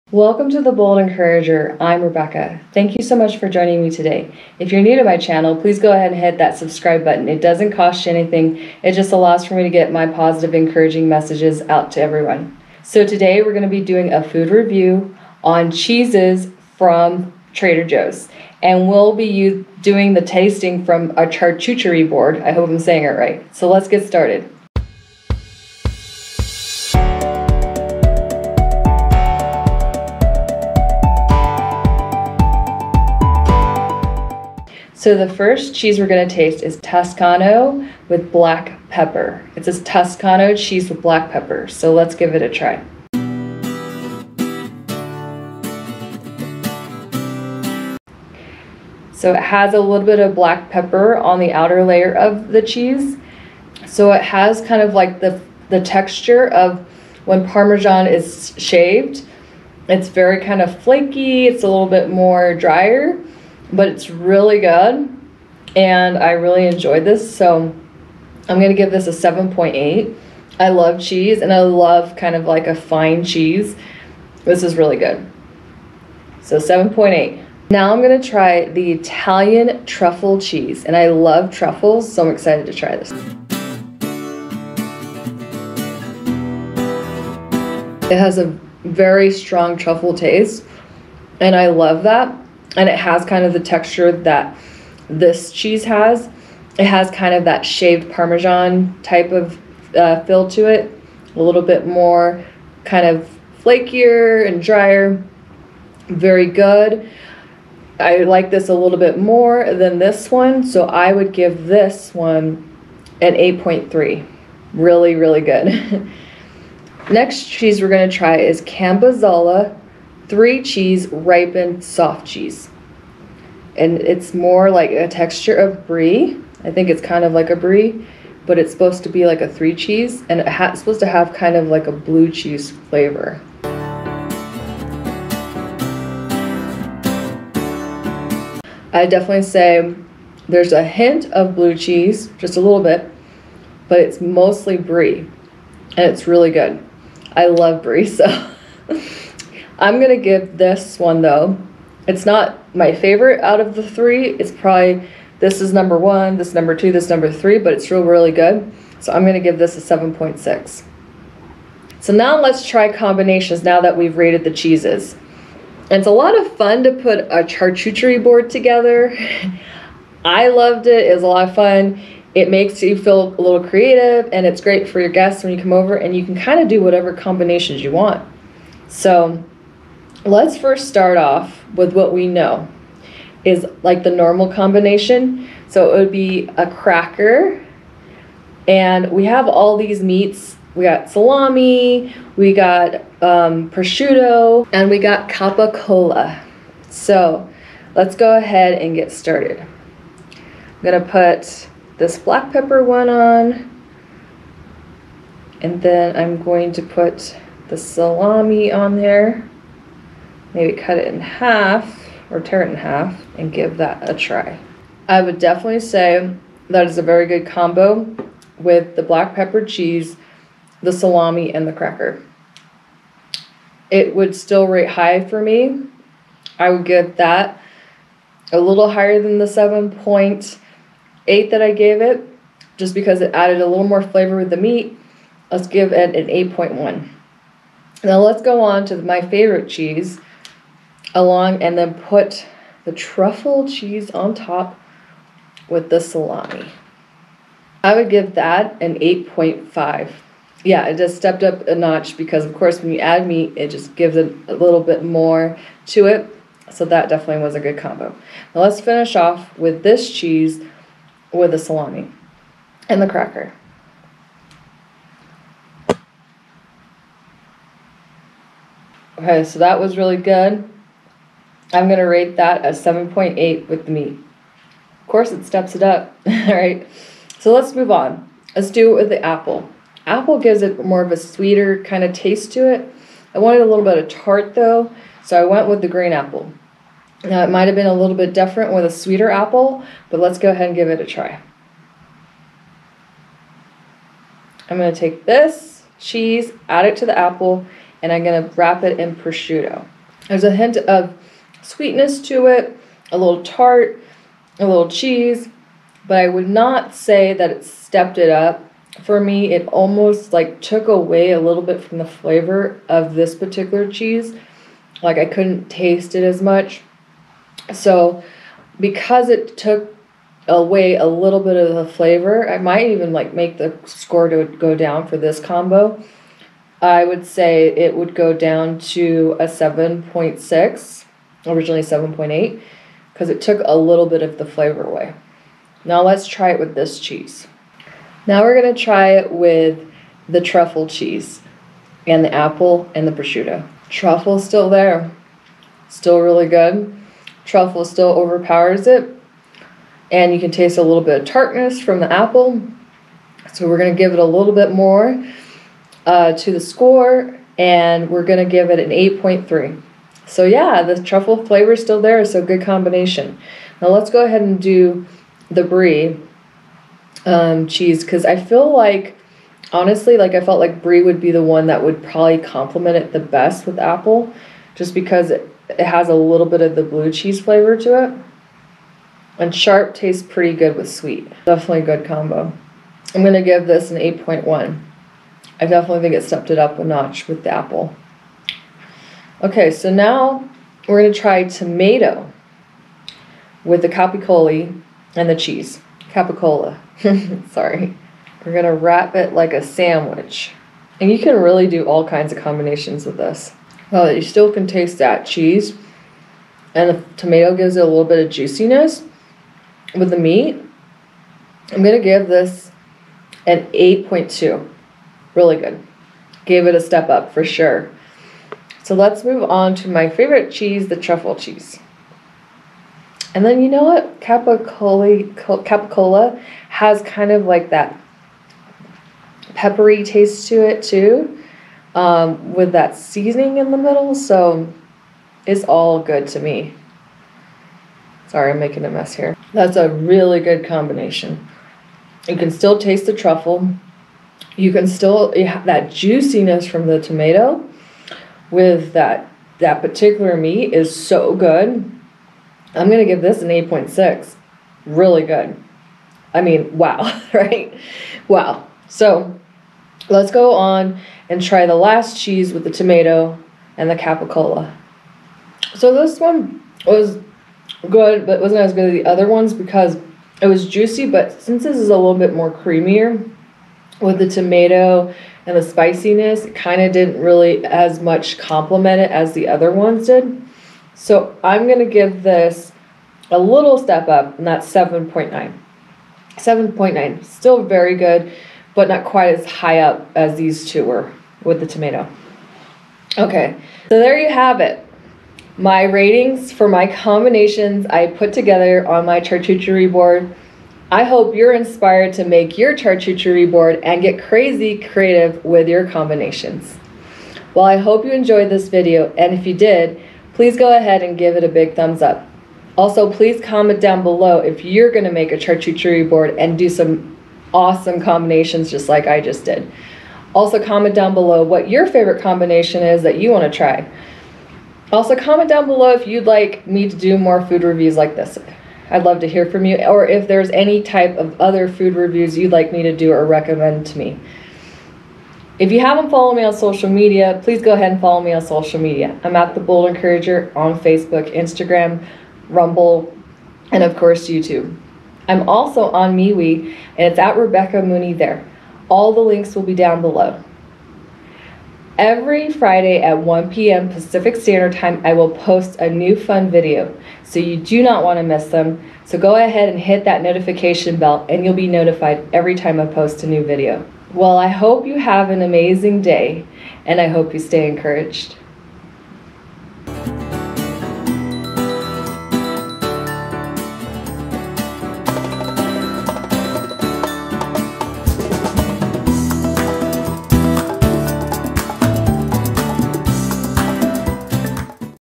Trader Joes Cheese and Charcuterie Board Combination Food Review
I will try three different cheeses from Trader Joes and rate them.  Then, I will try different combinations with the cheeses and rate them too.  The combinations will include cheese, crackers, deli meats, crackers, apples, and tomatoes. Please join me for this fun, creative and delicious food review.

Love, The Bold Encourager